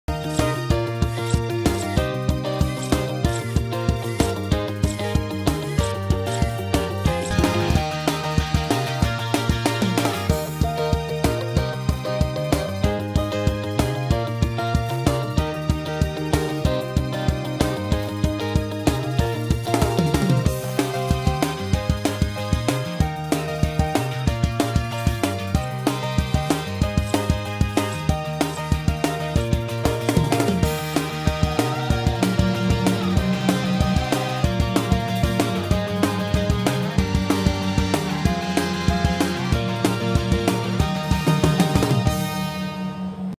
premade bag packing machine
premade bag packing machine for nuts ,candy,cookie ,dried beef cubes and so on .